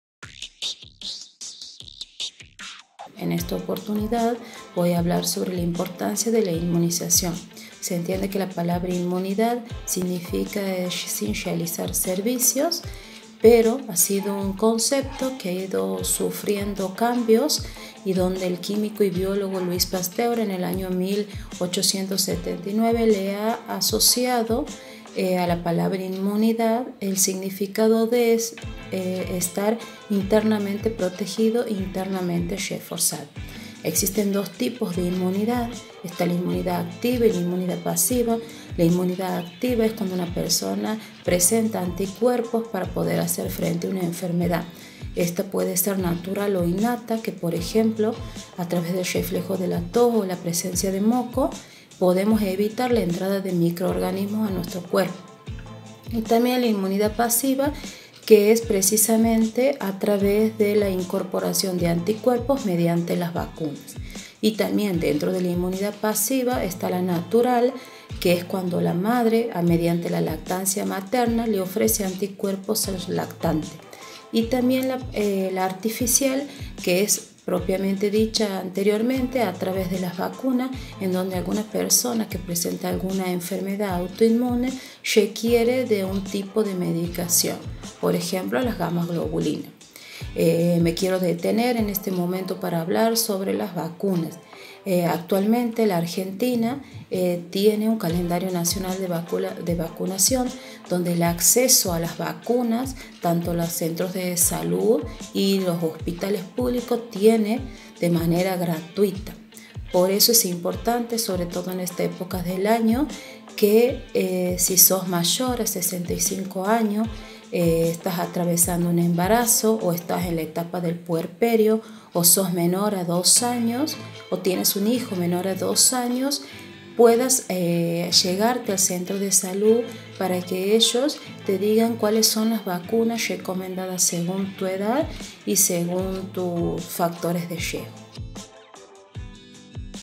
DESDE CASA Débora Arias
La docente de la licenciatura en enfermería, Débora Arias, nos habla sobre Inmunización y la importancia de las vacunas.